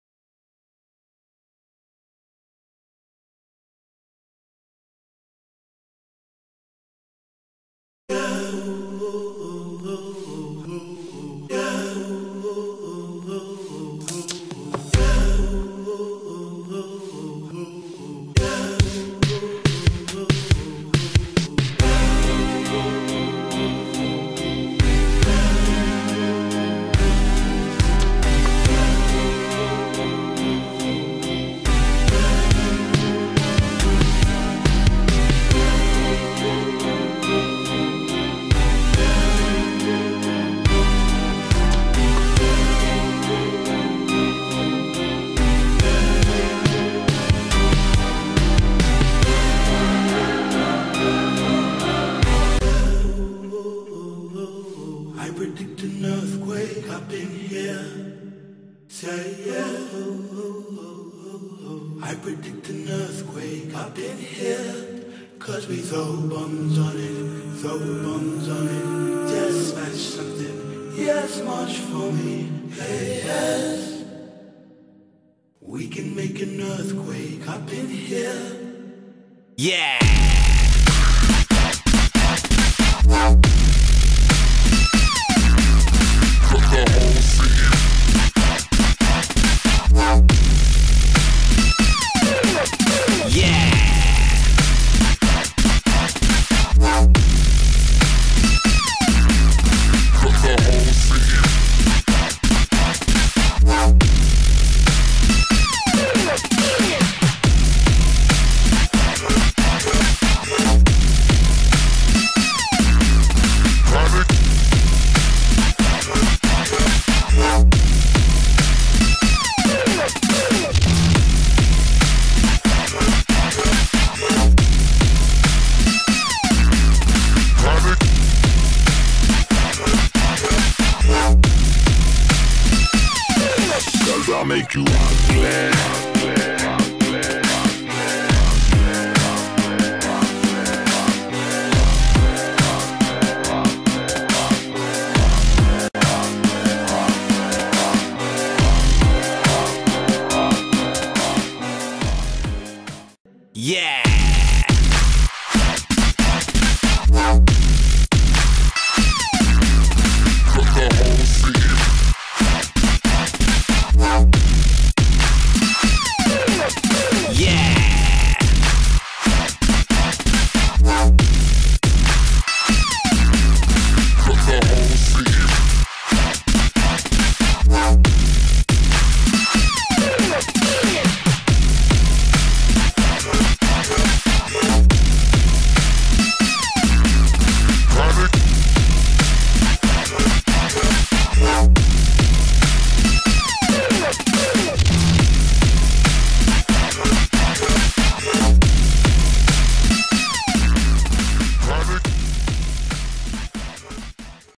Quake Style
Goin crazy freestylin with the industrial theme goin on ... Jumped some clothes on an headed out with my cam for an hour. This is what I came back with. Enjoy!